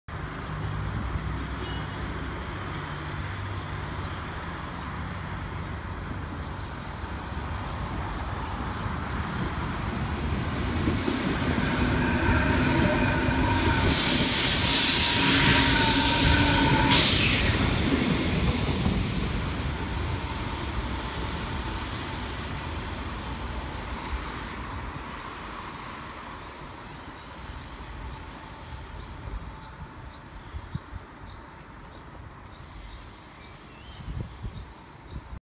Tren S-449 Media Distancia pasando por el apeadero de San Vicente del Raspeig.
Tren de Media Distancia S-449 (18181) procedente de Cuidad Real, pasando por el apeadero de San Vicent Centre (San Vicente Centro) por vía principal con destino Alicante Terminal.
Grabado el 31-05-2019 a las 20:31.

Recordar que seguimos publicando vídeos de archivo.